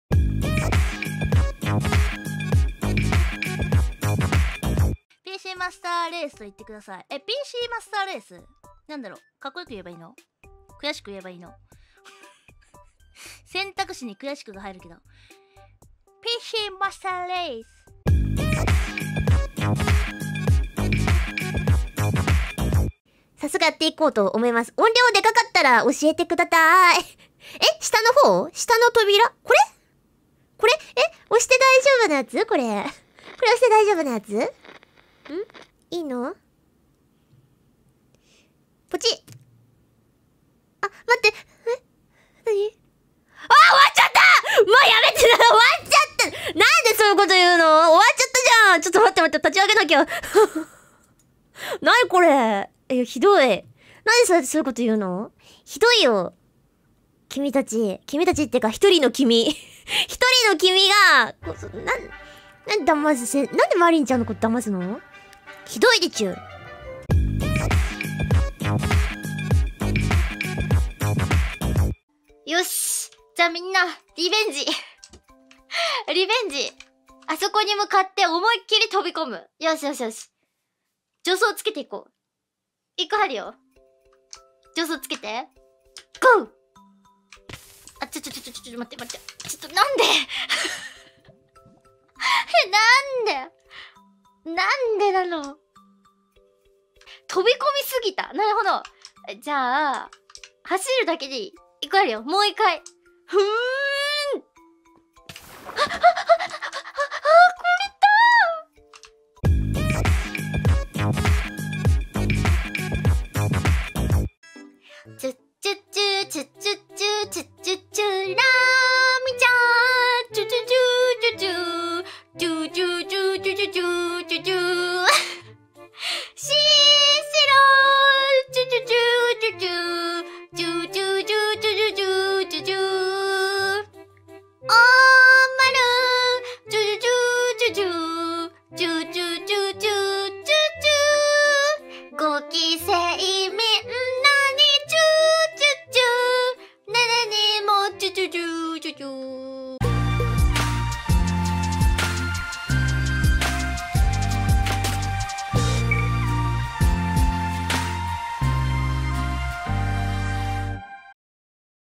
【Daily Hololive Highlight】Botan: PC Master Race, Marine: TRICKED, Nene: Revenge, Kissing ED【Eng Sub】
Daily Hololive Highlights. Compilation of funny and interesting Hololive vtubers moments. (23 Sept 2020)